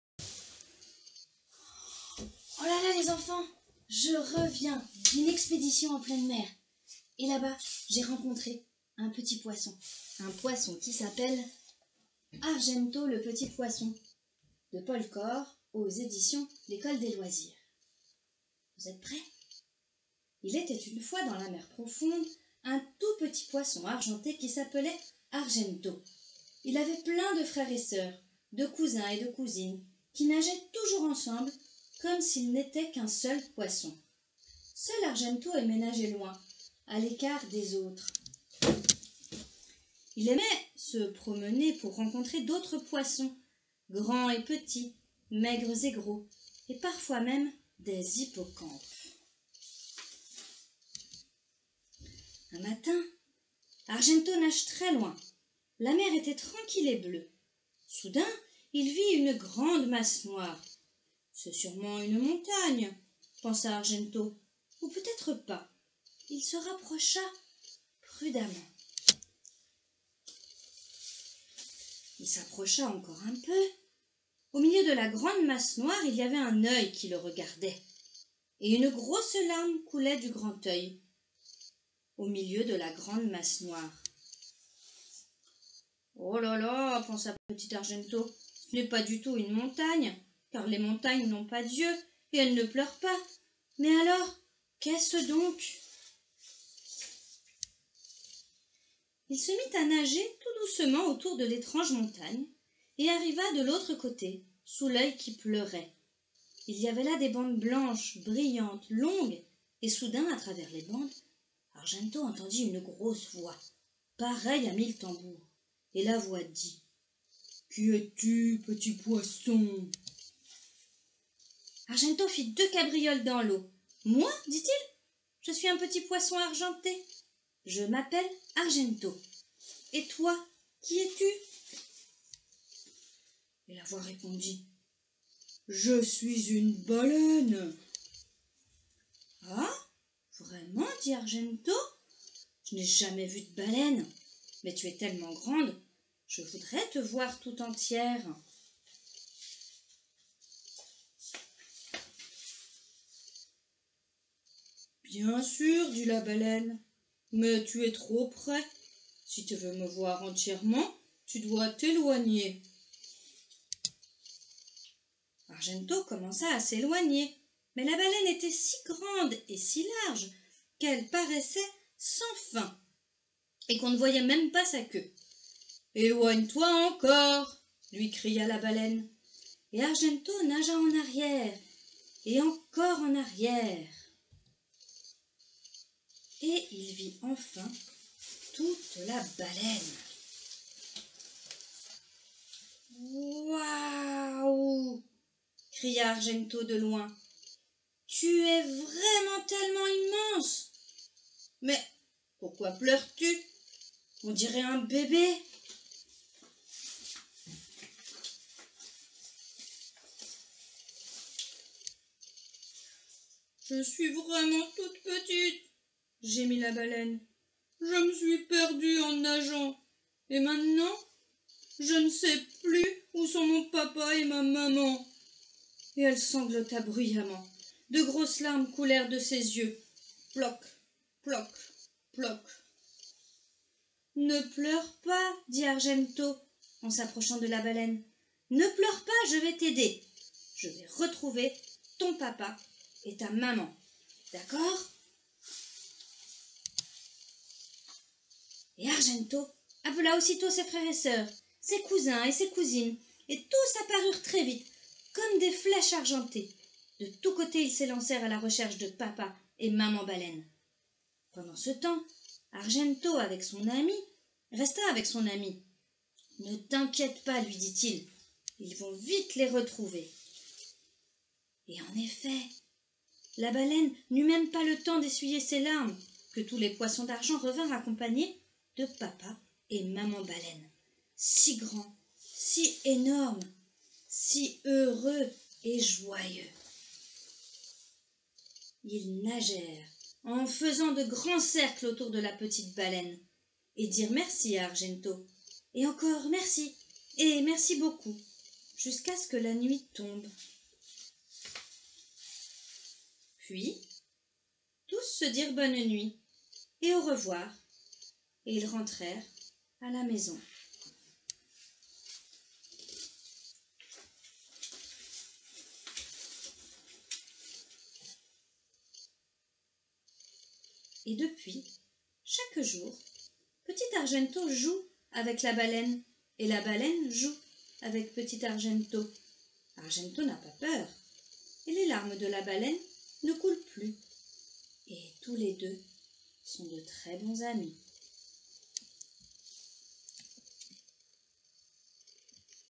"Argento Le petit poisson" - Une histoire de Paul Kor (l'école des loisirs)
Une belle histoire d'amitié et de famille pour finir cette semaine chez les poissons !